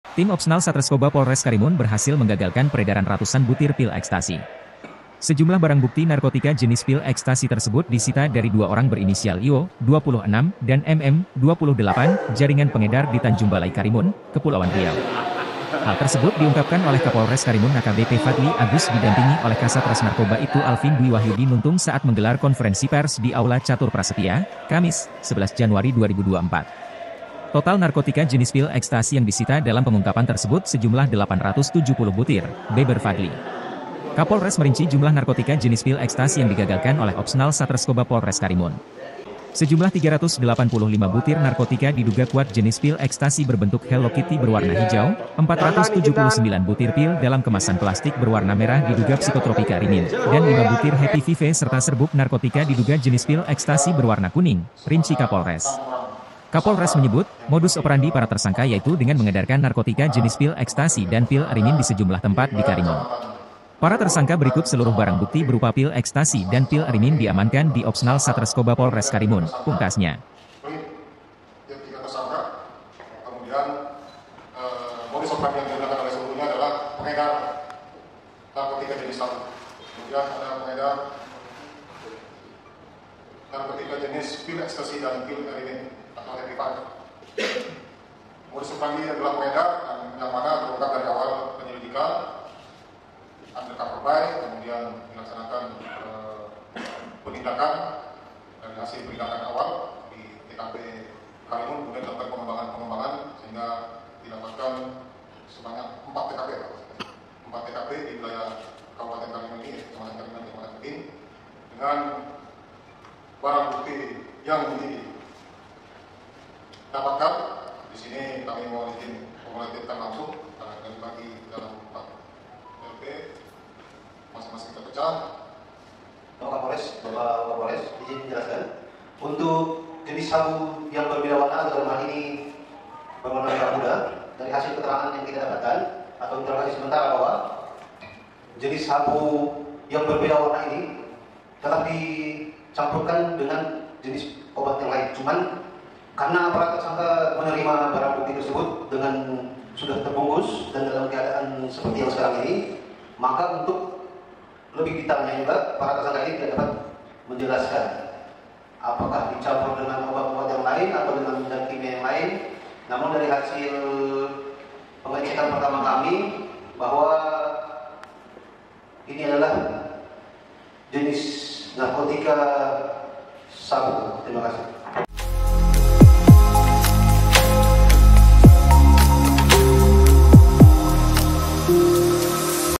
Satnarkoba Polres Karimun Ungkap Sabu Jenis Baru 2024
wartakepritv Satnarkoba Polres Karimun Ungkap Sabu Jenis Baru 2024